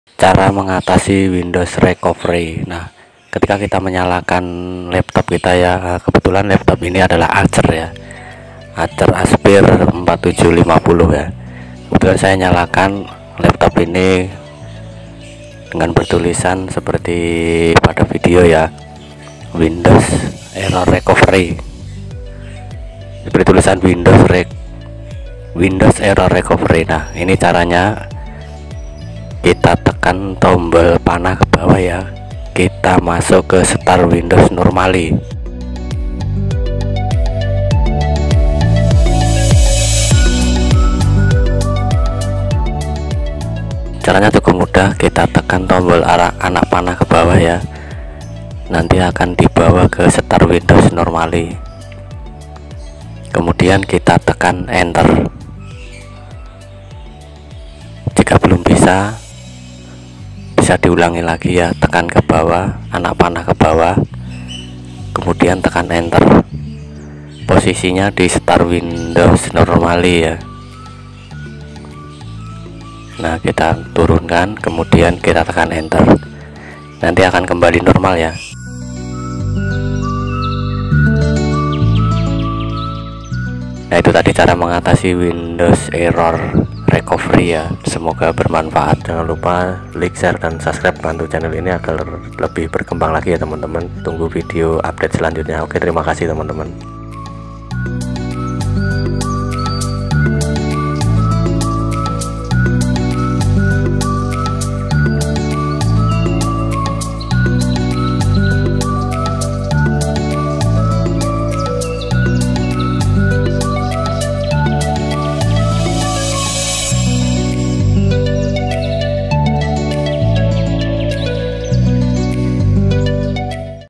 CARA MENGATASI WINDOWS ERROR RECOVERY 100% BERHASIL !!!
Bagi kalian yang bingung cara mengatasi Windows error recovery pada laptop acer Aspire 4750 dan merek lap top lain sebagainya.

Windows error di sebabkan karena beberapa hal, antara lainya adalah HDD status penyimpanan, juga di sebabkan oleh kesalahan pada bateray dan Arus listrik tiba tiba padam dalam keadaan terhubung dengan listrik tanpa UPS, dan kesalahan update windows.

Kebetulan pada video sebagai contoh Mengatasi Windows error recovery windows 7 64 bit atau juga windows 7 32 bit.